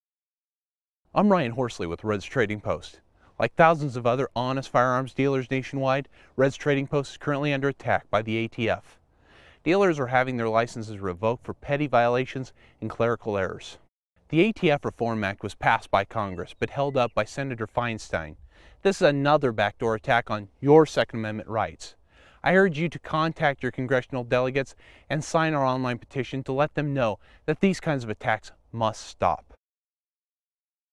Red's Trading Post Commercial II
This is one of two video commercials made for Red's Trading Post, a firearms business located in Twin Falls, Idaho.

Created as a message to the public alerting them to the destructive, wrongful actions of the Bureau of Alcohol, Tobacco, Firearms, and Explosives toward Red's and other firearms dealers around the country.